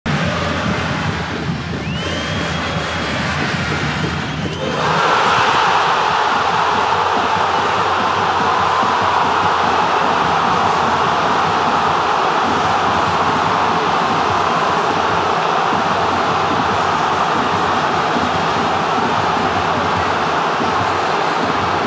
El gol de Juan Ignacio Méndez
Una bomba cayó en Liniers. Desde afuera del área, Juani Méndez clavó un misil y desató la euforia en el Amalfitani.